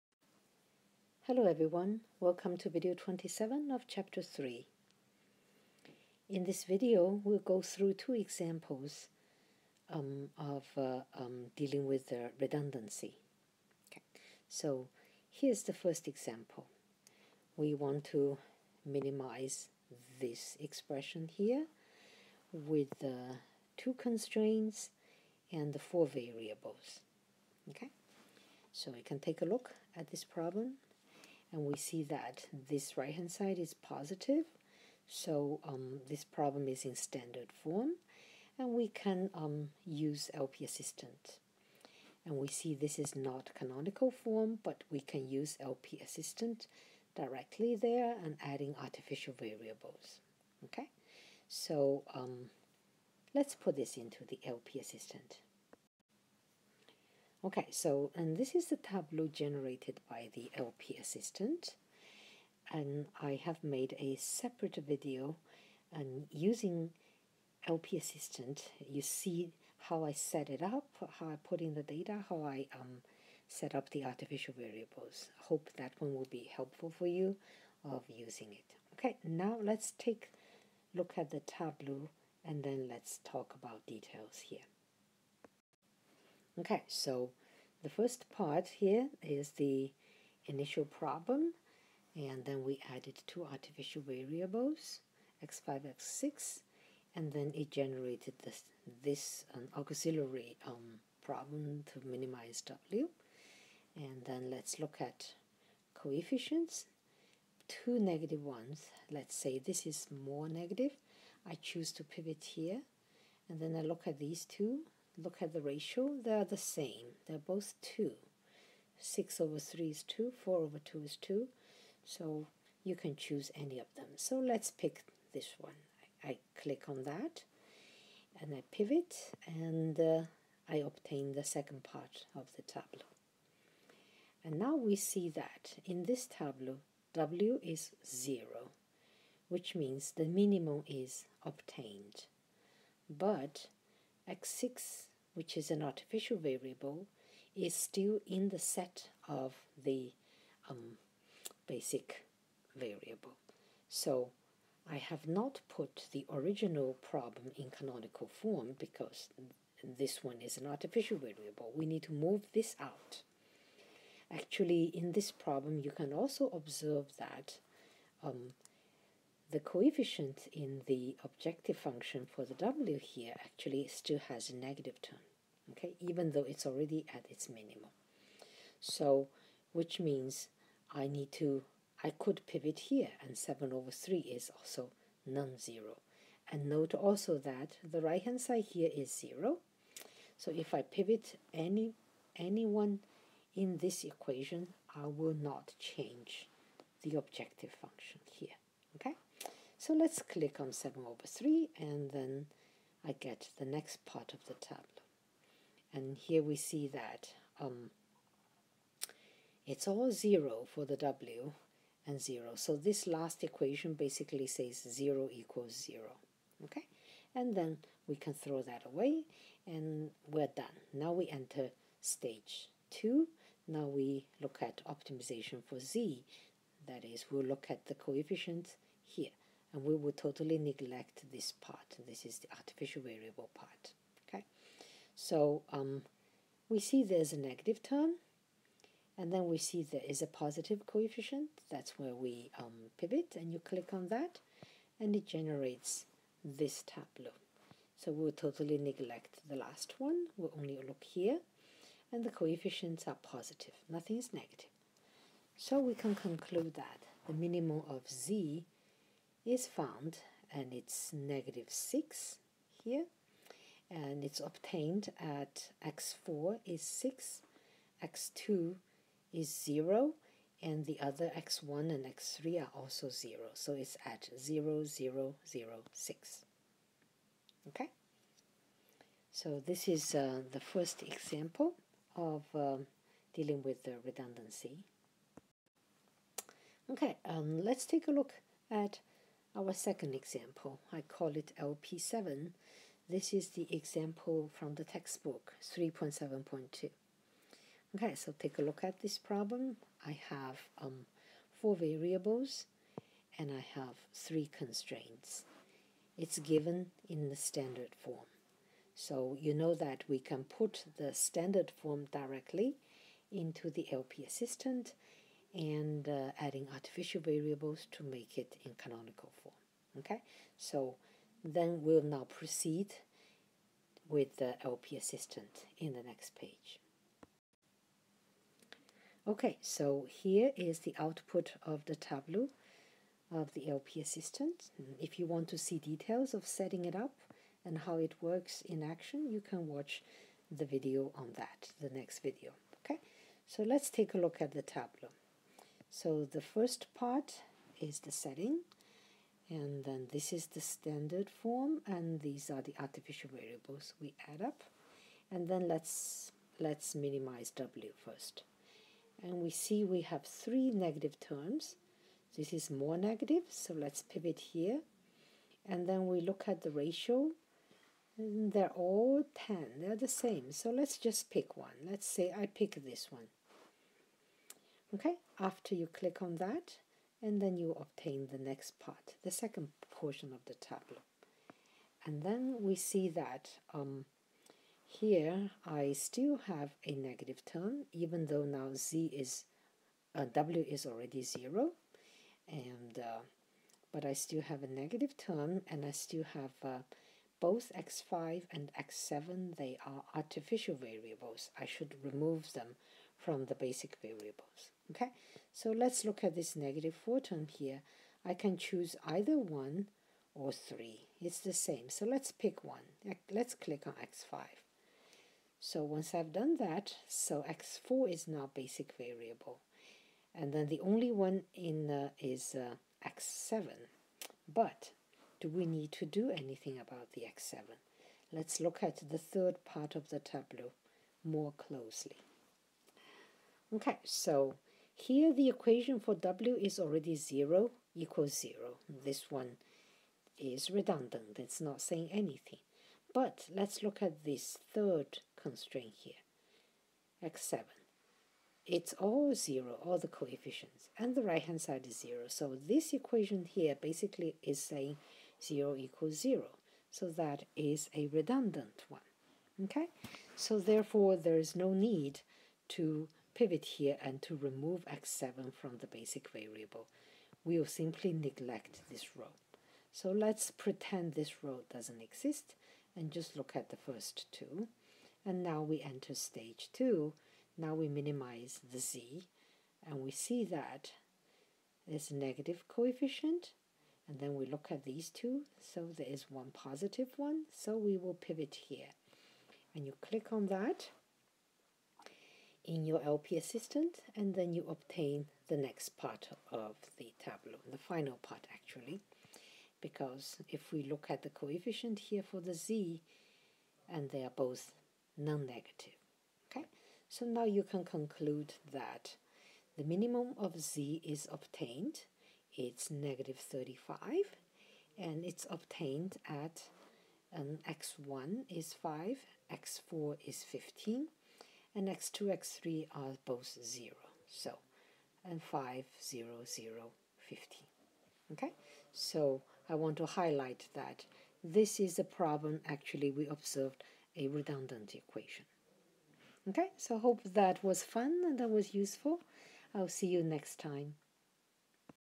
V3-27. Linear Programming. Redundant system. Example LP6.
Math 484: Linear Programming. Redundant system. Example LP6.
Wen Shen, 2020, Penn State University